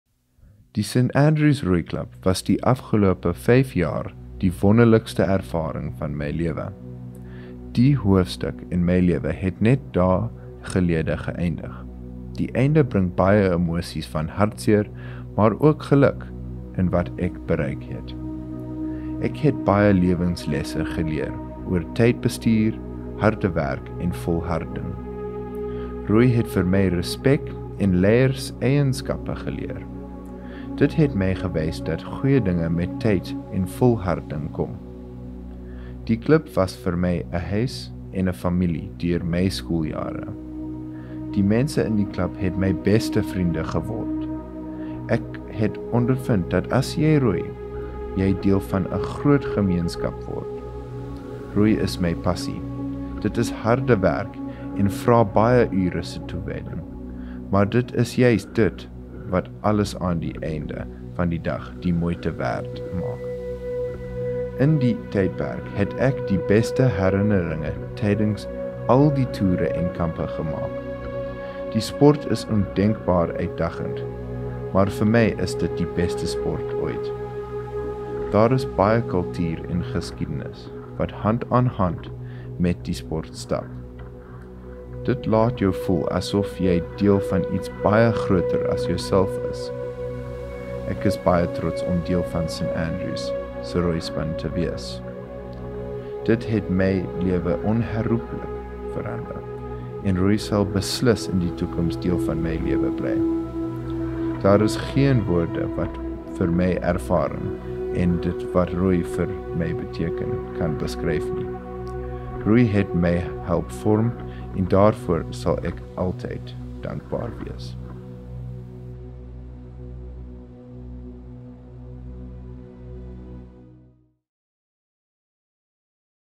Scott Moorcroft - Roei
Scott Moorcroft’s Afrikaans poem “Roei” formed part of Speech Day presentations, under the title: “The Things we Love and Leave Behind in our Journey”